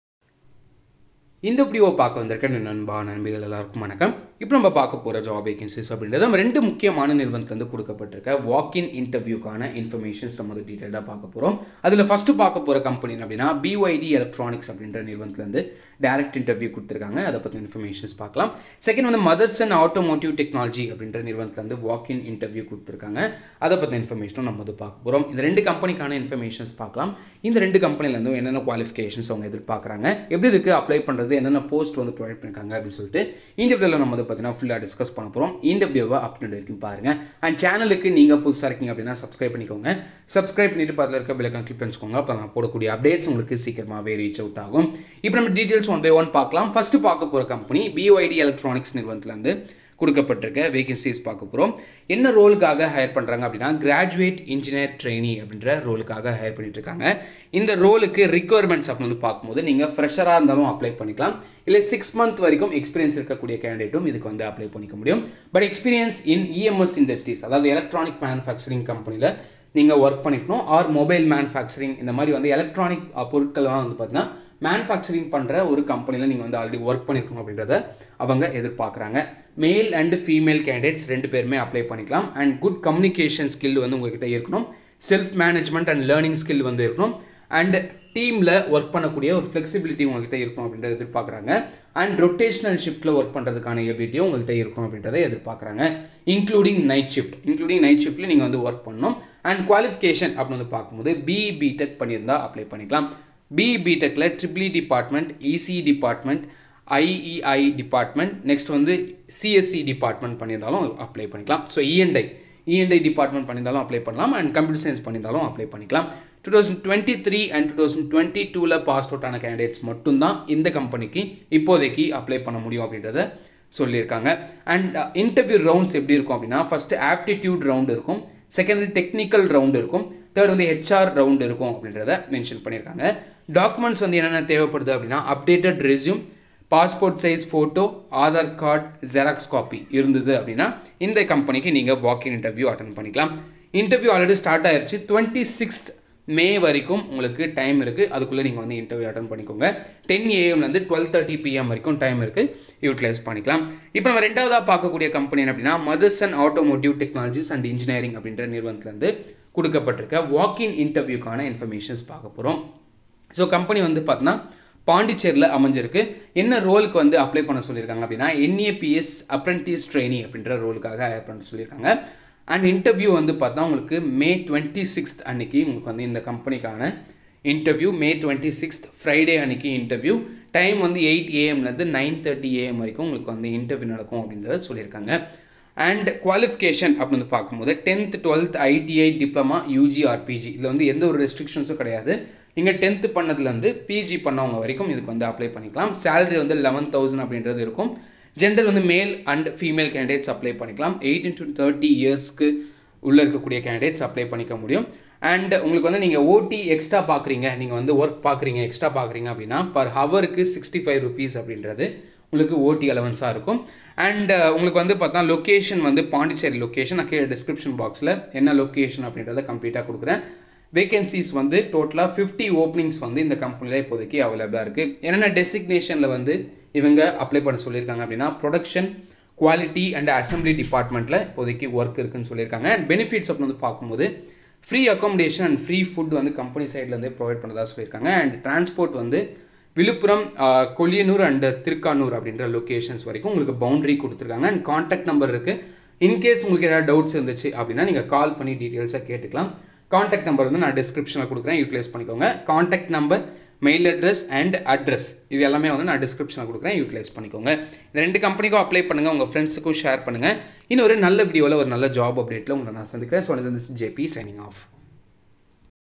Direct Interview🔥 BYD Electronics & Motherson Automotive ltd-Any Degree/Diploma/ITI/+2/10th Eligible
Apply Details👇

Accenture Hiring Freshers 💥 TATA Autocomp - Walkin interview / Bachelor's Degree & PG Eligible

👉BYD Electronics - Direct Interview

Location - R-19 , New 38, 6th Main Road, Anna Nagar, Chennai, Tamil Nadu 600040

👉Motherson Automotive Technology - Walkin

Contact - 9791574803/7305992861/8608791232
Location - WMC7+76R, National Highway 45A, Thiruvandarkoil, puducherry 605107